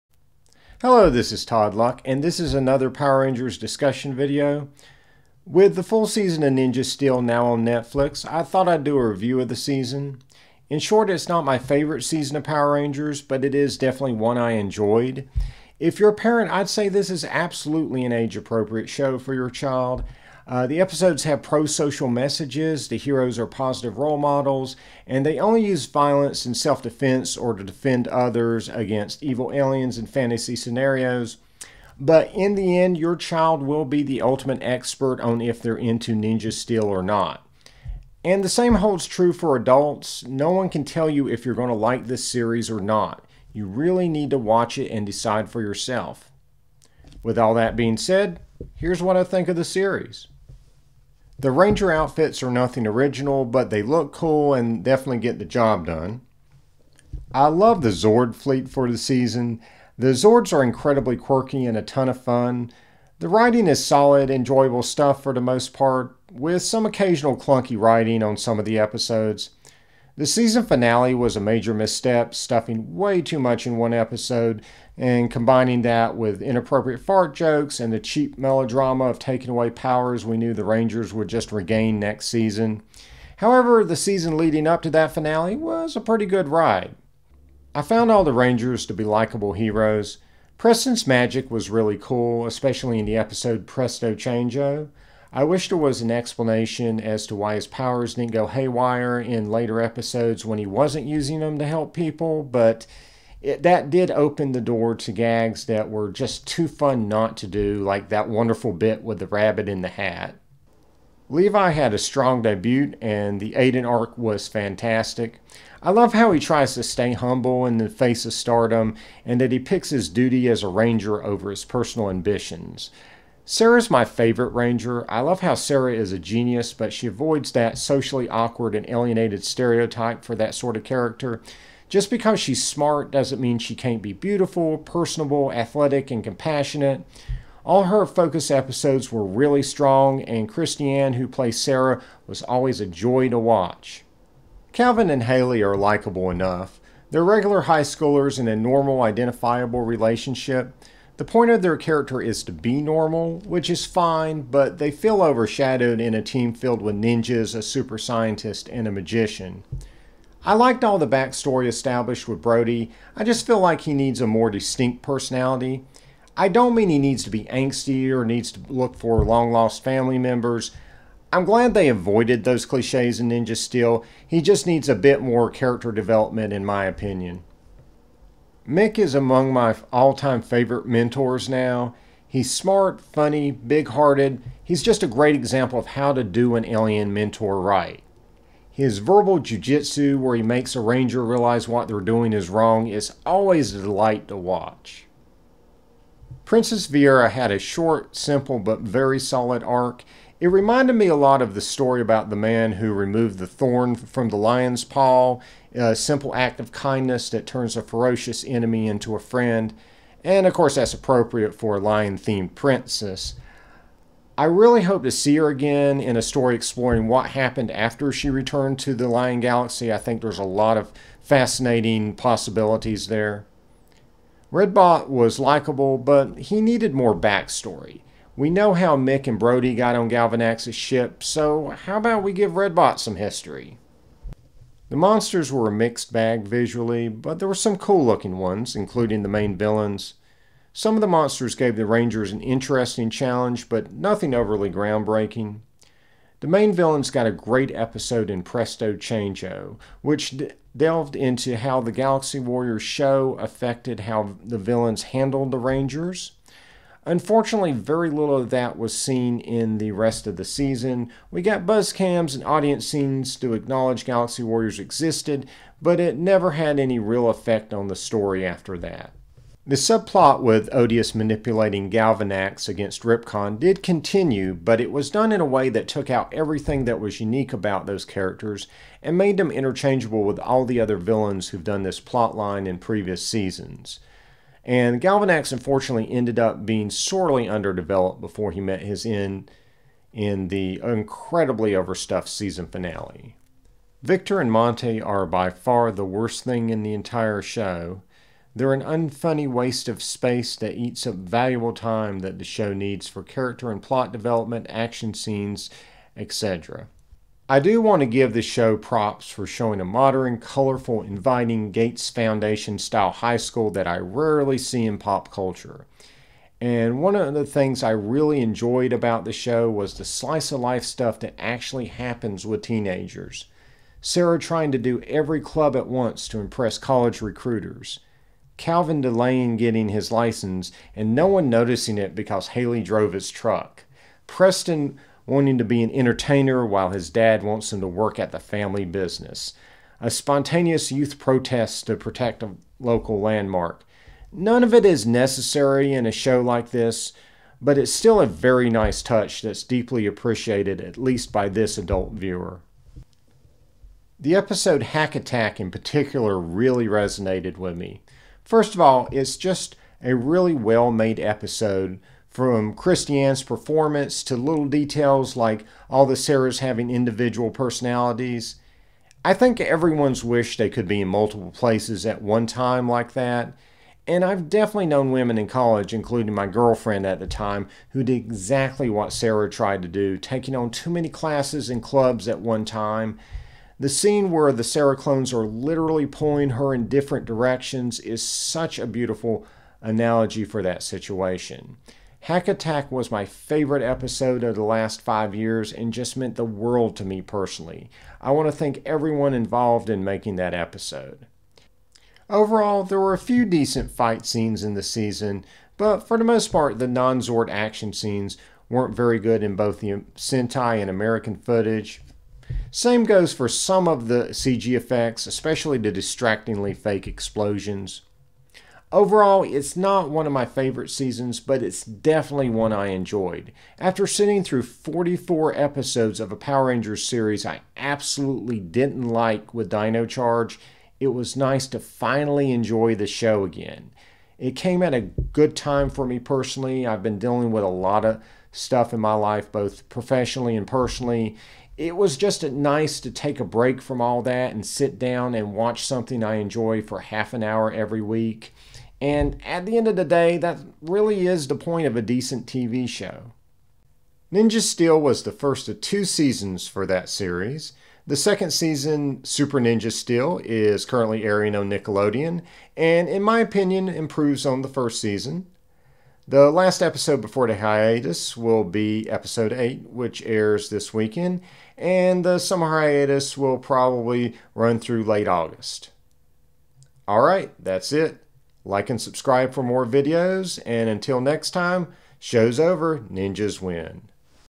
Power Rangers Ninja Steel Review
A review of the first season of Power Rangers Ninja Steel.